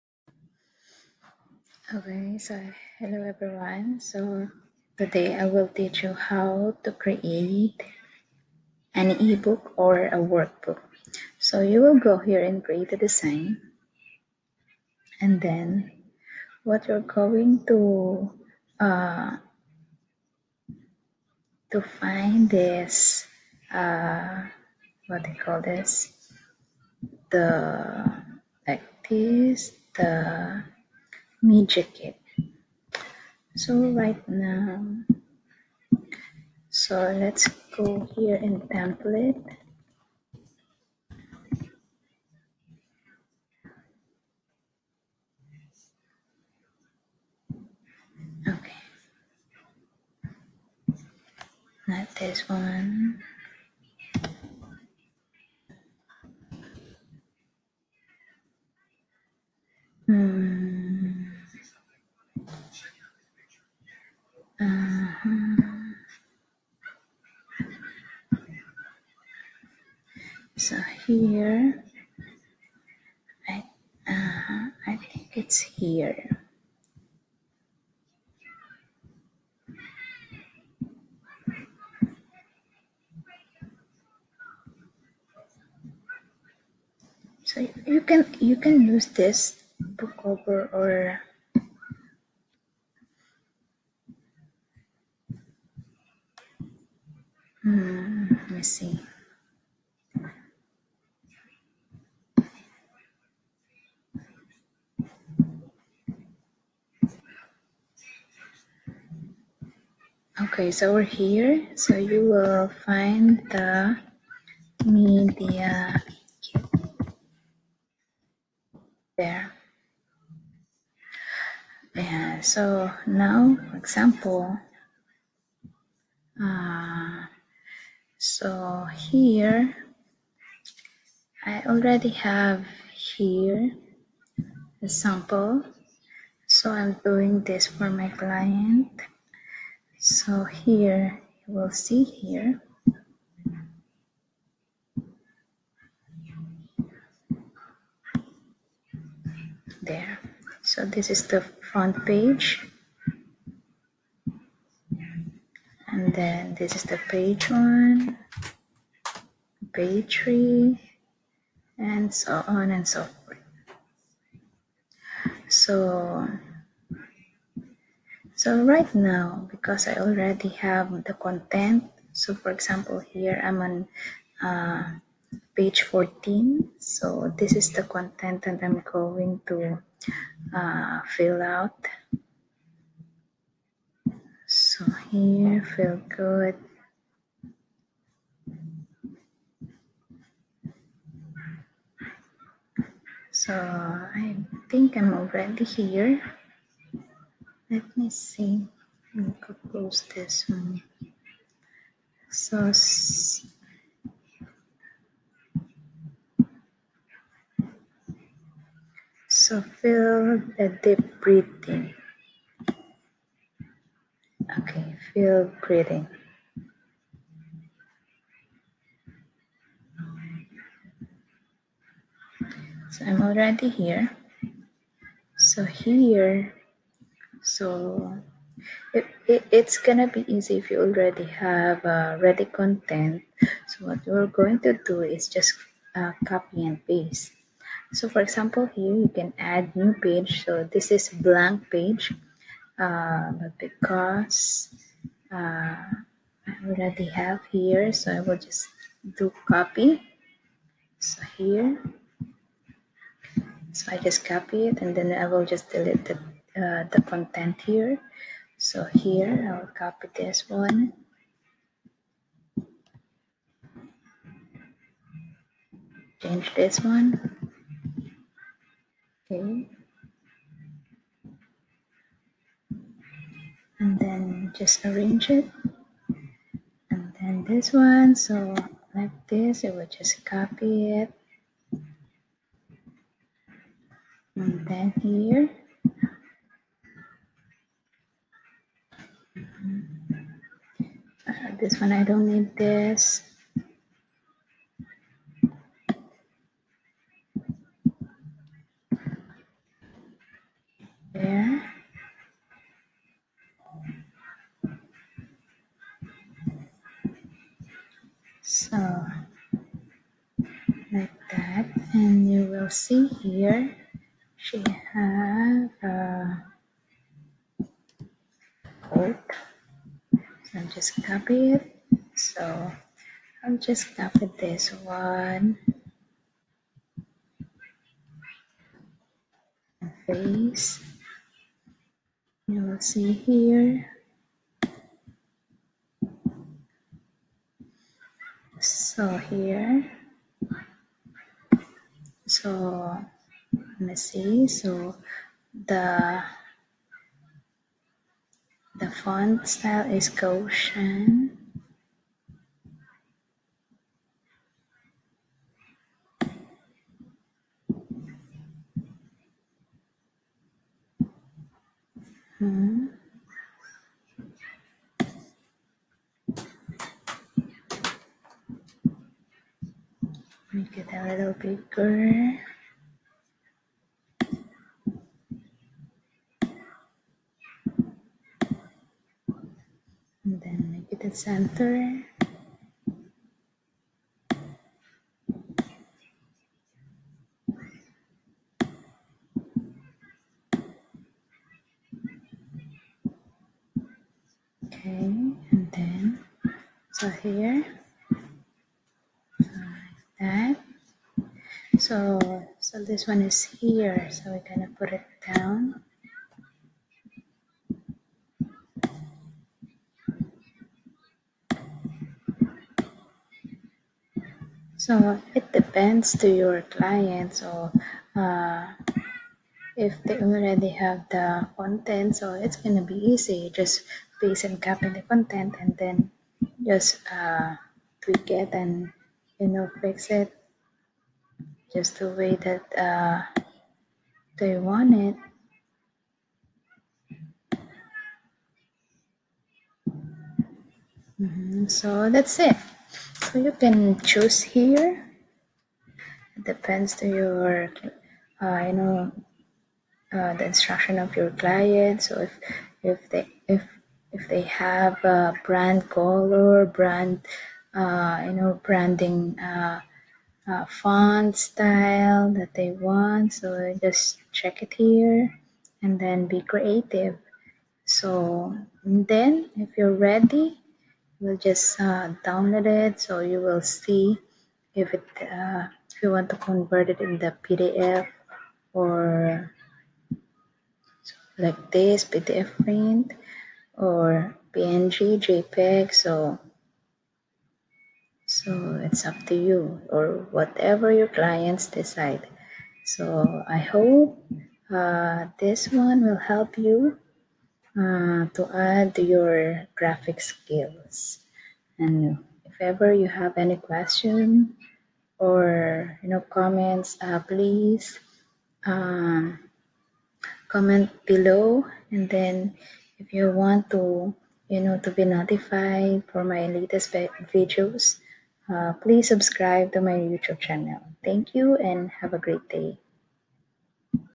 How to create a E-book in Canva
One of the Tasks of a VA is to design Pinterest images, Facebook images, Twitter images, Instagram images, LinkedIn images, and Youtube images.